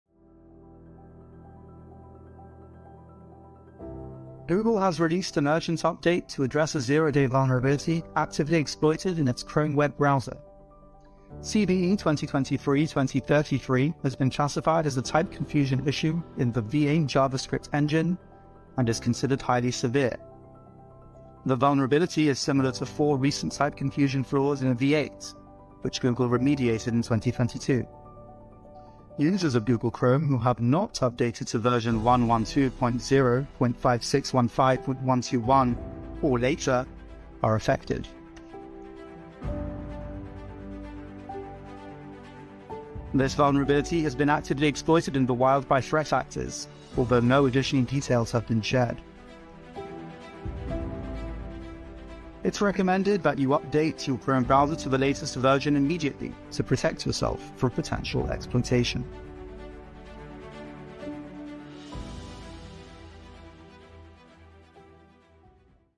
How to fix CVE 2023 2033 in Google Chrome
CVE-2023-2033 affects the Google Chrome browser and has been exploited in the wild. Here's everything you need to know.